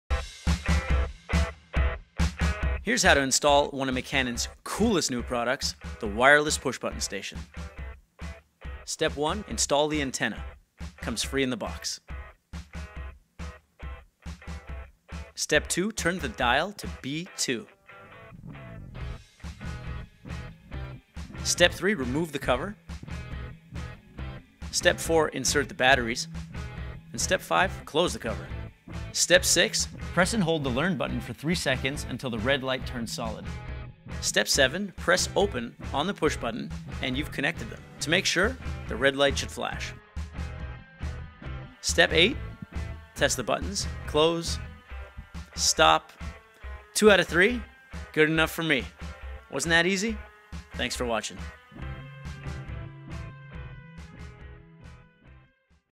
How-To: Set up a Wireless Nice Garage Door Push Button Station
Here's a step-by-step video on how to set up a wireless Nice garage door push button station.